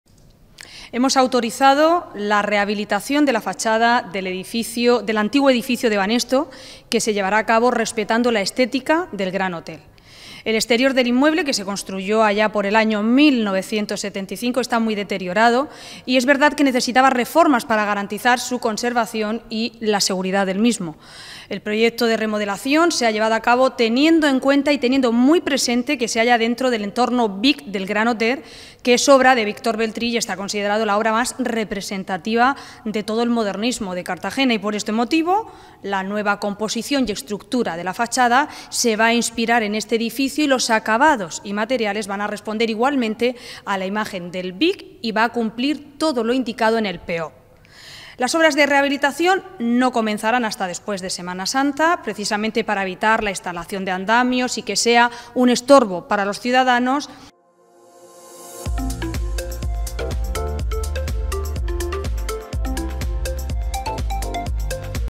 El edificio Banesto se adaptará a la estética del Gran Hotel
La concejalía de Urbanismo, que dirige la vicealcaldesa, Ana Belén Castejón, ha autorizado la rehabilitación de la fachada del edificio Banesto, que se llevará a cabo respetando la estética del Gran Hotel.

El exterior del inmueble, que se construyó en 1975, está muy deteriorado y necesitaba reformas para garantizar su conservación y su seguridad. De hecho, se han producido desprendimientos de cascotes en los años 2010, 2017 y 2019. “El proyecto de remodelación se ha llevado a cabo teniendo en cuenta que se halla dentro del entorno BIC del Gran Hotel, que es obra de Víctor Beltrí y está considerado la obra más representativa del modernismo en Cartagena”, ha señalado la vicealcaldesa y concejala de Urbanismo, Ana Belén Castejón.

Por este motivo, la nueva composición y estructura de la fachada se inspira en este edificio y los acabados y materiales responden igualmente a la imagen del BIC y cumple lo indicado en el PEOPCH. El presupuesto de estas obras ronda los 180.000 euros.

“Las obras de rehabilitación no comenzarán hasta después de Semana Santa para evitar la instalación de andamios. En este entorno se están rehabilitando más edificios, como los situados en las calles Carmen, plaza San Sebastián y Jara,  lo que contribuirá a mejorar la imagen del centro histórico”, ha precisado Castejón.

El edificio, que tiene fachadas a las calles Honda, Jara y Plaza de San Sebastián, se enmarca en el régimen de inadecuación urbanística, porque no está adaptado a las determinaciones del Plan Especial de Ordenación y Protección del Conjunto Histórico (PEOPCH) en lo que respecta a su volumen edificado, a la ocupación de fondos y a las alturas permitidas.